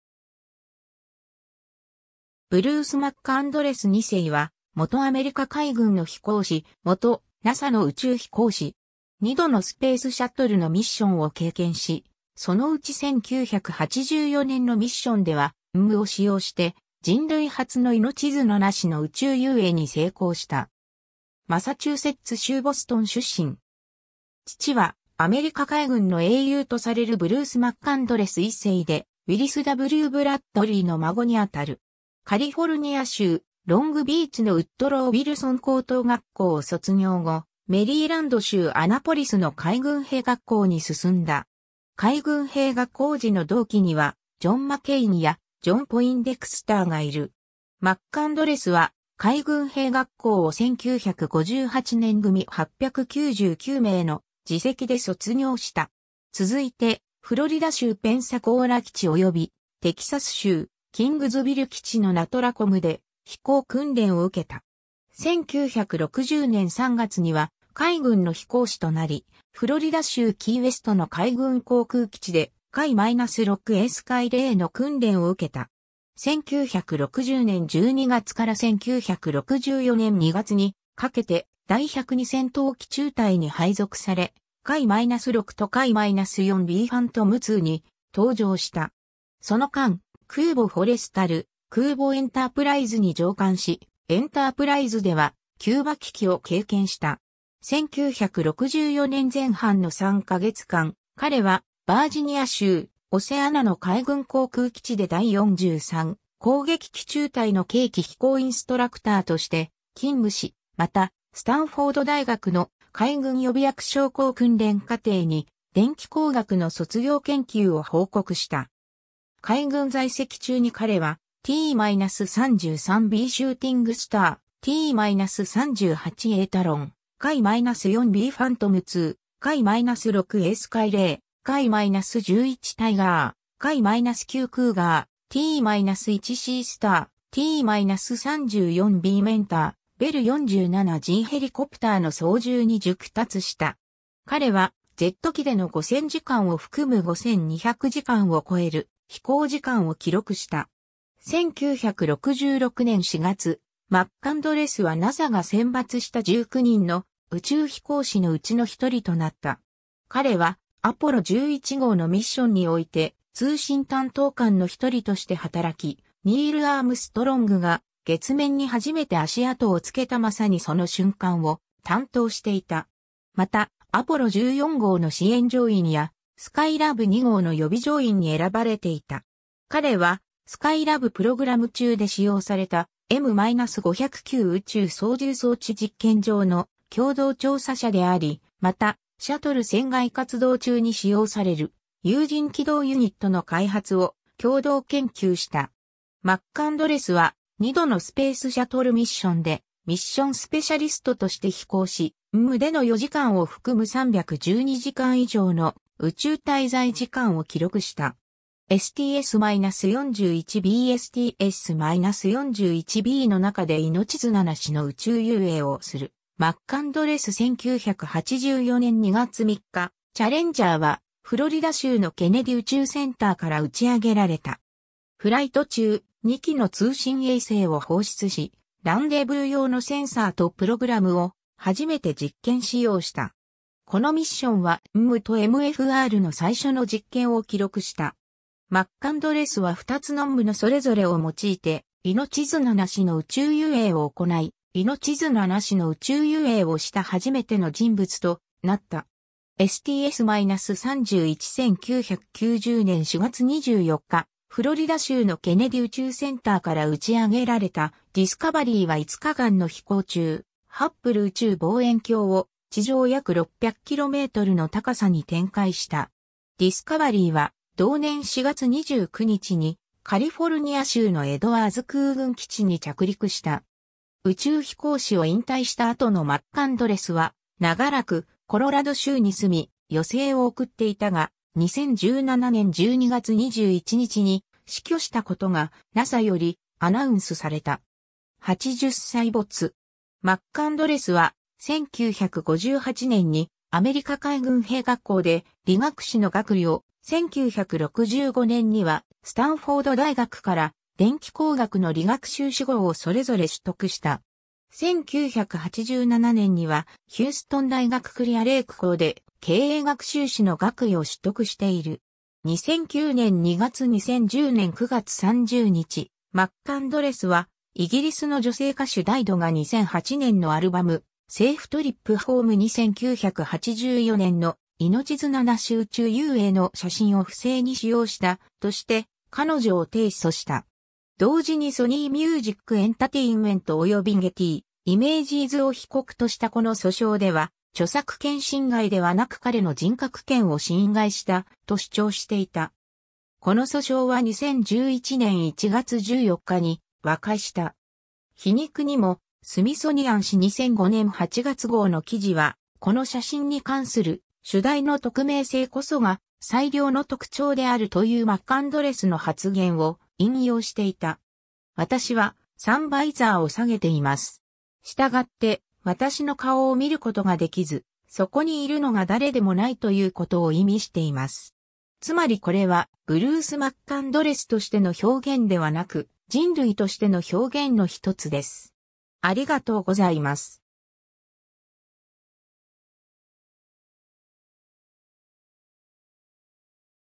ブルース・マッカンドレス2世
ブルース・マッカンドレス2世（Bruce McCandless II, 1937年6月8日 - 2017年12月21日）は、元アメリカ海軍の飛行士、元NASAの宇宙飛行士。
 2度のスペースシャトルのミッションを経験し、そのうち1984年のミッションではMMU（有人機動ユニット、Manned Maneuvering Unit）を使用して、人類初の命綱なしの宇宙遊泳に成功した。
 マサチューセッツ州ボストン出身。
父はアメリカ海軍の英雄とされるブルース・マッカンドレス1世で、ウィリス・W・ブラッドリーの孫にあたる。
 カリフォルニア州ロングビーチのウッドロウ・ウィルソン高等学校を卒業後、メリーランド州アナポリスの海軍兵学校に進んだ。
海軍兵学校時の同期にはジョン・マケインやジョン・ポインデクスターがいる。
 マッカンドレスは、海軍兵学校を1958年組899名の次席で卒業した。
続いて、フロリダ州ペンサコーラ基地及びテキサス州キングズヴィル基地のNATRACOM（海軍訓練航空団）で飛行訓練を受けた。
 1960年3月には海軍の飛行士となり、フロリダ州キーウェストの海軍航空基地でF-6A スカイレイの訓練を受けた。
 1960年12月から1964年2月にかけて、第102戦闘機中隊（VF-102）に配属され、F-6とF-4BファントムIIに搭乗した。
その間、空母フォレスタル、空母エンタープライズに乗艦し、エンタープライズではキューバ危機を経験した。
 1964年前半の3か月間、彼はヴァージニア州オセアナの海軍航空基地で第43攻撃機中隊（VA-43）の計器飛行インストラクターとして勤務し、またスタンフォード大学の海軍予備役将校訓練課程に電気工学の卒業研究を報告した。
 海軍在籍中に彼は、T-33B シューティングスター、T-38A タロン、F-4B ファントムII、F-6A スカイレイ、F-11 タイガー、F-9 クーガー、T-1 シースター、T-34B メンター、ベル47Gヘリコプターの操縦に熟達した。
 彼はジェット機での5,000時間を含む5,200時間を超える飛行時間を記録した。
 1966年4月、マッカンドレスはNASAが選抜した19人の宇宙飛行士の内の1人となった。
 彼はアポロ11号のミッションにおいて通信担当官の1人として働き、ニール・アームストロングが月面に初めて足跡を付けたまさにその瞬間を担当していた。
またアポロ14号の支援乗員や、スカイラブ2号の予備乗員に選ばれていた。
 彼は、スカイラブ・プログラム中で使用されたM-509宇宙操縦装置実験上の共同調査者であり、またシャトル船外活動（EVA）中に使用される有人機動ユニット（MMU）の開発を共同研究した。
マッカンドレスは、2度のスペースシャトルミッション（STS-41-B及びSTS-31）でミッション・スペシャリストとして飛行し、MMUでの4時間を含む312時間以上の宇宙滞在時間を記録した。
 STS-41-B STS-41-Bの中で命綱なしの宇宙遊泳をするマッカンドレス 1984年2月3日、チャレンジャーはフロリダ州のケネディ宇宙センターから打ち上げられた。
フライト中、2機の通信衛星を放出し、ランデブー用のセンサーとプログラムを初めて実験使用した。
 このミッションは、MMUとMFR（Manipulator Foot Restraint）の最初の実験を記録した。
マッカンドレスは2つのMMUのそれぞれを用いて命綱なしの宇宙遊泳を行い、命綱なしの宇宙遊泳をした初めての人物となった。
 STS-31 1990年4月24日、フロリダ州のケネディ宇宙センターから打ち上げられたディスカバリーは5日間の飛行中、ハッブル宇宙望遠鏡を地上約600kmの高さに展開した。
ディスカバリーは同年4月29日にカリフォルニア州のエドワーズ空軍基地に着陸した。
 宇宙飛行士を引退した後のマッカンドレスは、長らくコロラド州に住み、余生を送っていたが、2017年12月21日に死去したことがNASAよりアナウンスされた。
80歳没。
 マッカンドレスは、1958年にアメリカ海軍兵学校で理学士の学位を、1965年にはスタンフォード大学から電気工学の理学修士号をそれぞれ取得した。...